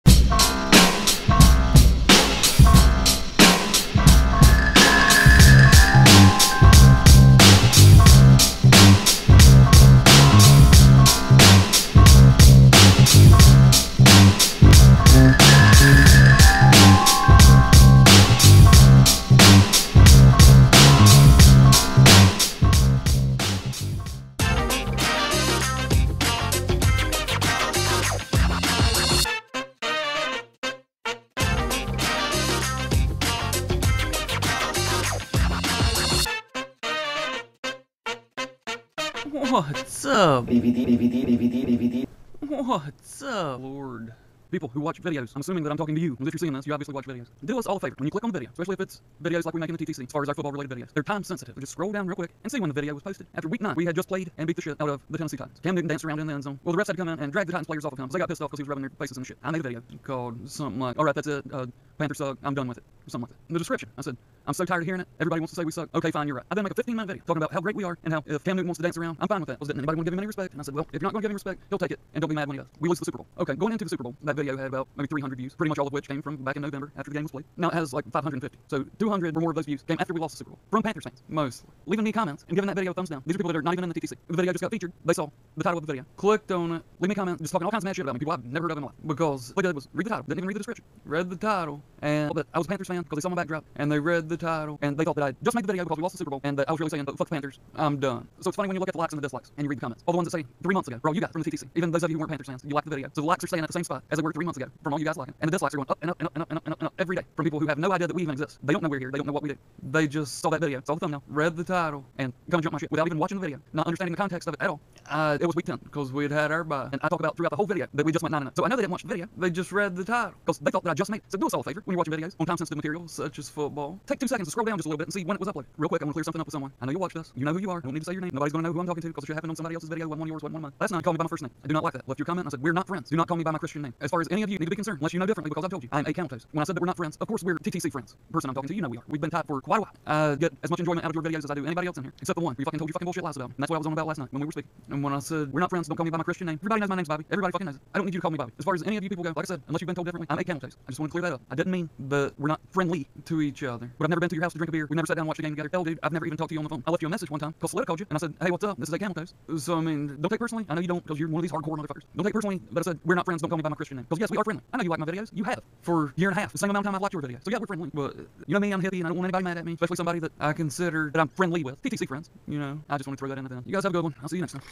Making 8cameltoes watchable
you probably thought it was impossible to sit thru an entire 8cameltoes video, but folks, i made it possible for us ALL.